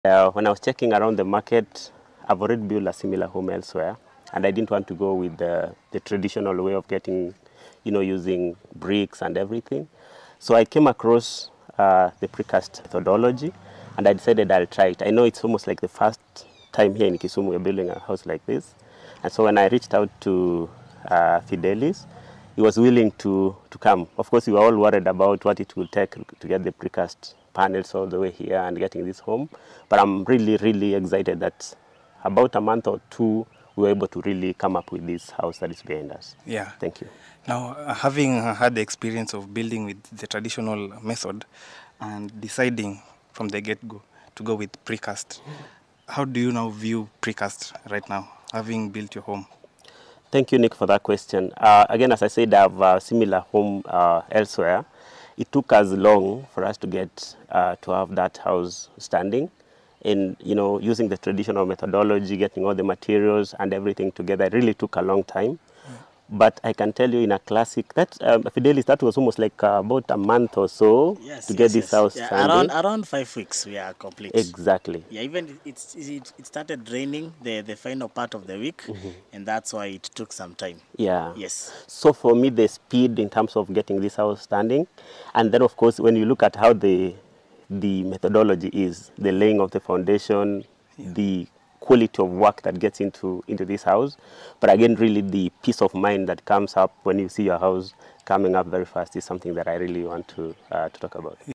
HOW FAST IT IS CONSTRUCTING WITH PRECAST
Precast brings an edge over construction with bricks by reducing project timelines and cost significantly as the elements are manufactured off site and delivered on site
Engage us on your construction project: